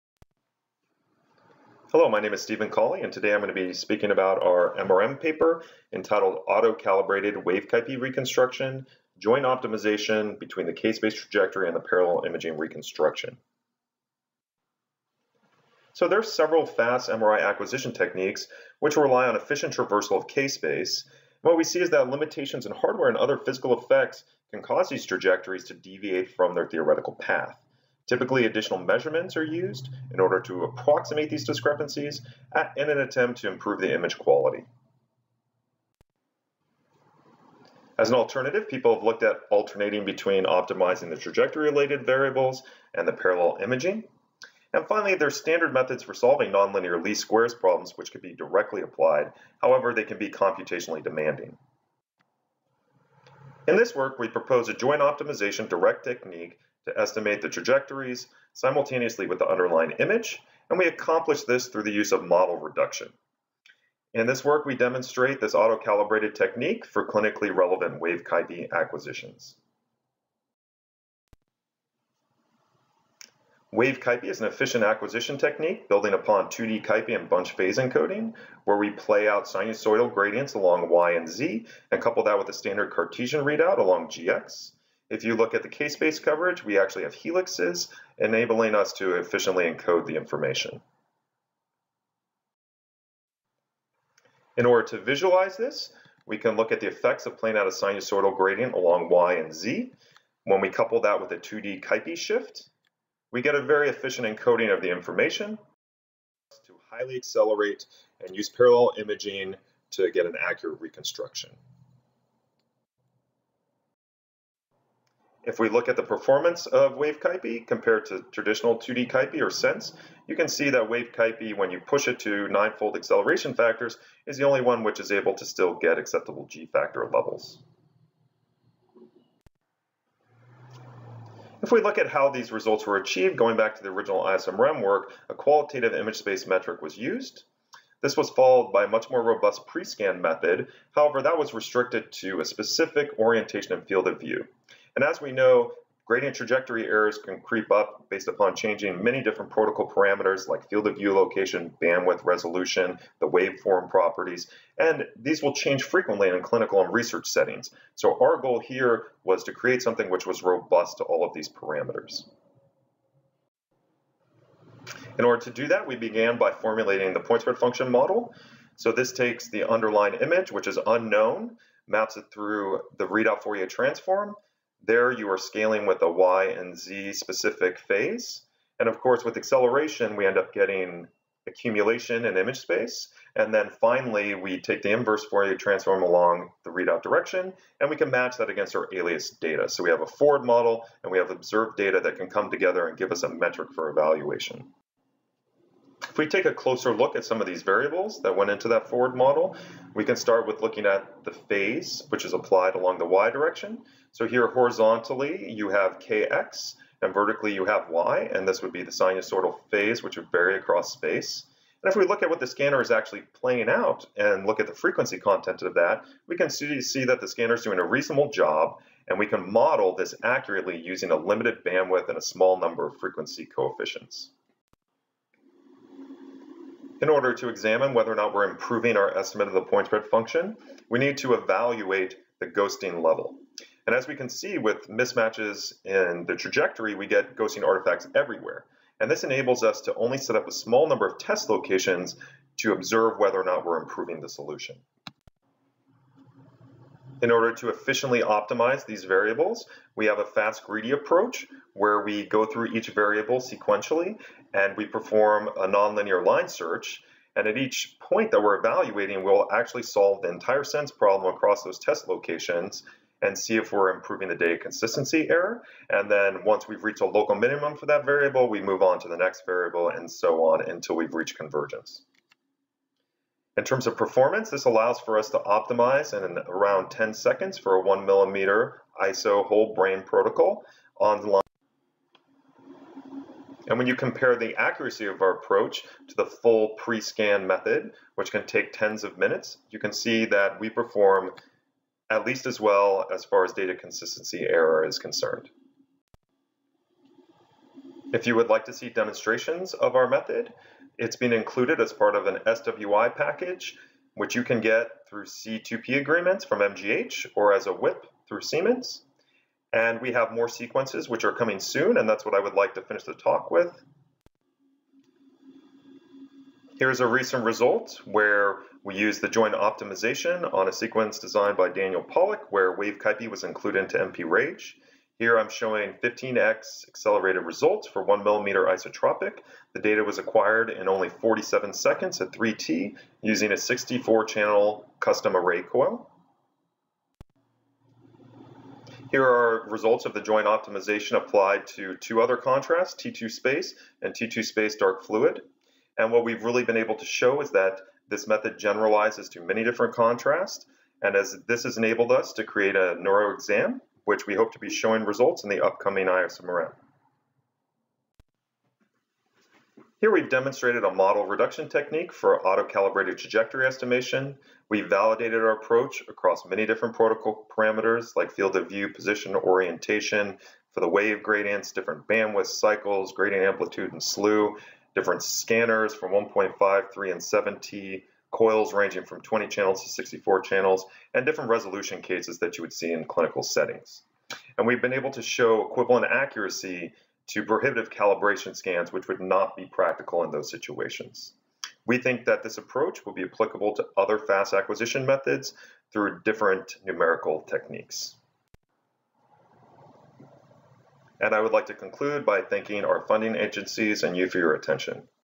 Stephen Cauley: Autocalibrated wave-CAIPI reconstruction
An audioslide accompanying the MRM Editor's pick for September 2017, entitled "Autocalibrated wave-CAIPI reconstruction; Joint optimization of k-space trajectory and parallel imaging reconstruction" presented by Stephen Cauley from Department of Radiology, Harvard Medical School, Boston, Massachu-setts, USA.